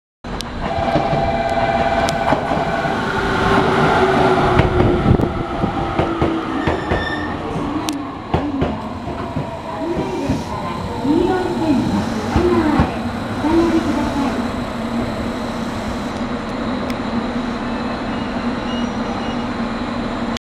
普通 黒崎行(1624H) 817系運用[V2003](2両編成) 直方駅到着
★撮影場所:JR直方駅
★列車名:
★列車番号: 1624H
★編成:2両編成[V2003](817系)
★停車駅(鹿児島本線･福北ゆたか線[篠栗線･筑豊本線]経由)
博多―Hakata
吉塚―Yoshizuka
柚須―Yusu
原町―Harumachi
長者原―Cyoujyabaru
門松―Kadomatsu
篠栗―Sasaguri
筑前山手―Chikuzen-Yamate
城戸南蔵院前―Kidonanzouin-mae
九郎原―Kuroubaru
筑前大分―Chikuzen-Daibu
桂川―Keisen
天道―Tentou
飯塚―Iizuka
新飯塚―Shin-Iizuka
浦田―Urata
鯰田―Namazuda
小竹―Kotake
勝野―Katsuno
直方―Nogata
新入―Shinnyu
筑前植木―Chikuzen-Ueki
鞍手―Kurate
筑前垣生―Chikuzen-Habu
中間―Nakama
東水巻―Higashi-Mizumaki
折尾―Orio
陣原―Jinnoharu
黒崎―Kurosaki